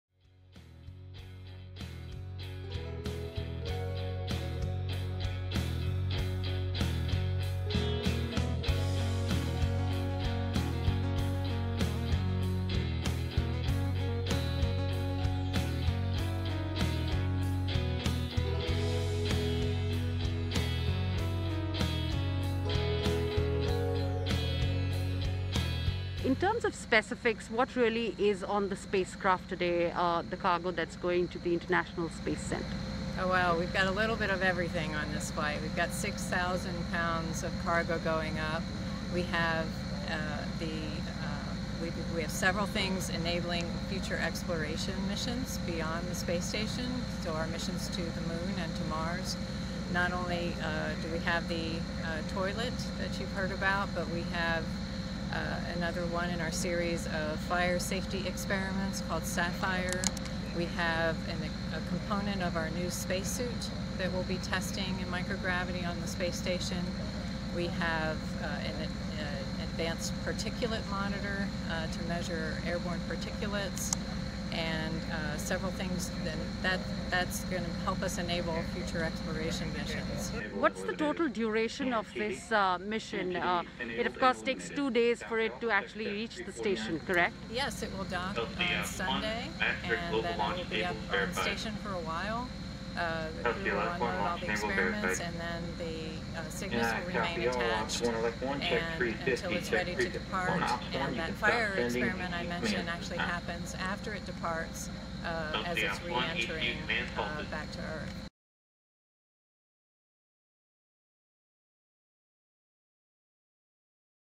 NASA launches cargo spacecraft named after Kalpana Chawla
United States of America's Northrop Grumman's Cygnus Spacecraft, named after Astronaut Kalpana Chawla was launched from National Aeronautics and Space Administration's Wallops Flight Facility.